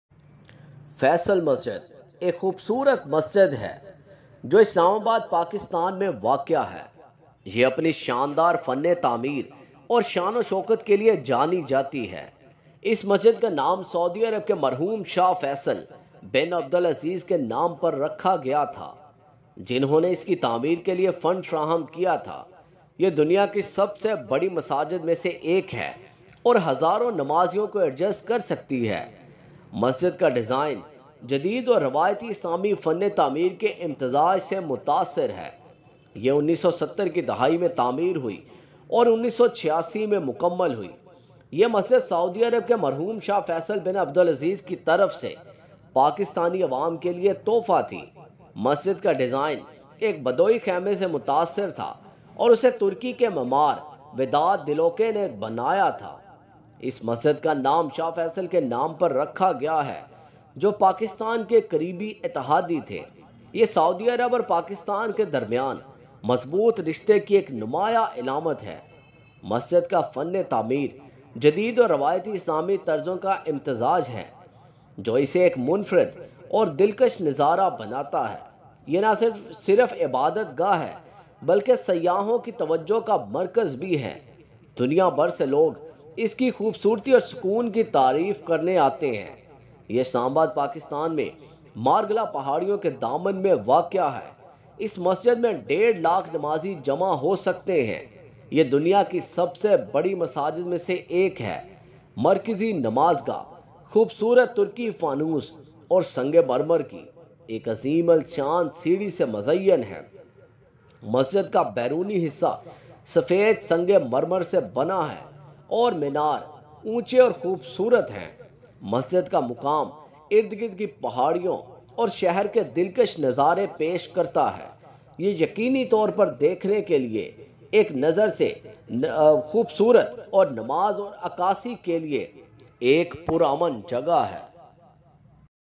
History of Faisal Mosque || Interesting information about beautiful and largest Mosque.......
Faisal Mosque! It's a stunning landmark in Islamabad, Pakistan. It's one of the largest mosques in the world and its architecture is incredible. The mosque was a gift from Saudi Arabia and was completed in 1986. Its design is a beautiful blend of modern and traditional elements. The mosque can accommodate around 100,000 worshippers, and its main prayer hall is breathtaking. If you ever get a chance to visit Islamabad, I highly recommend checking out Faisal Mosque. It's a sight you won't want to miss! 🕌.